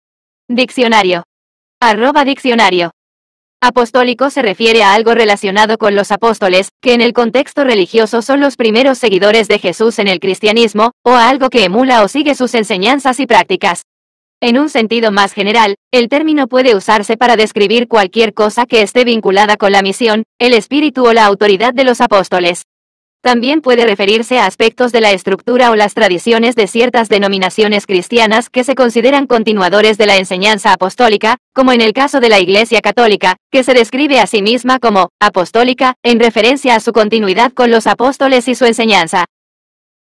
Apostólico Significado En Español
Apostólico se refiere a algo relacionado con los apóstoles, que en el contexto religioso son los primeros seguidores de Jesús en el cristianismo, o a algo que emula o sigue sus enseñanzas y prácticas. En un sentido más general, el término puede usarse para describir cualquier cosa que esté vinculada con la misión, el espíritu o la autoridad de los apóstoles. También puede referirse a aspectos de la estructura o las tradiciones de ciertas denominaciones cristianas que se consideran continuadores de la enseñanza apostólica, como en el caso de la Iglesia Católica, que se describe a sí misma como "apostólica" en referencia a su continuidad con los apóstoles y su enseñanza.